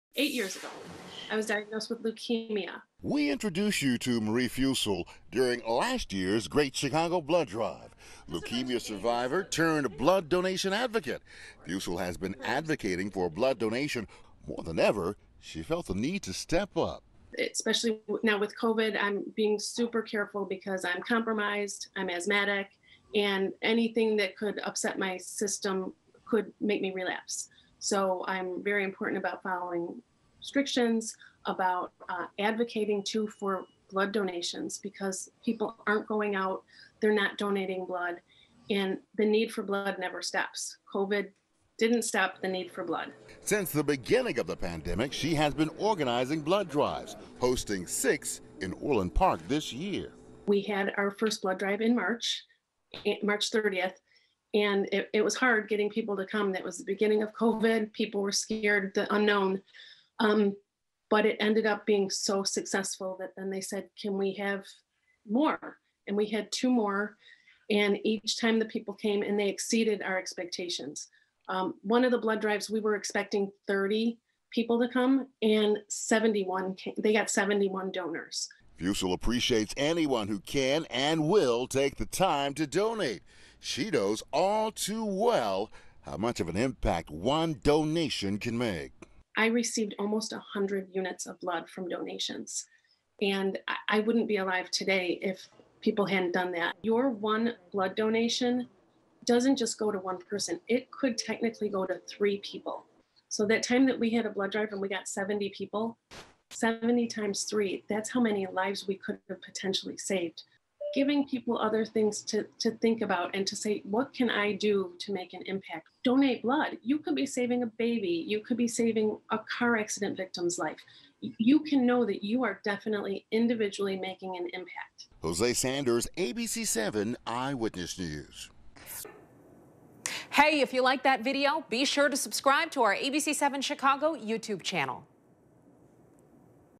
Cancer survivor mobilizes for blood donations druring pandemic despite personal risk
Marie Fuesel, a leukemia survivor, is going above and beyond to help save lives. FULL STORY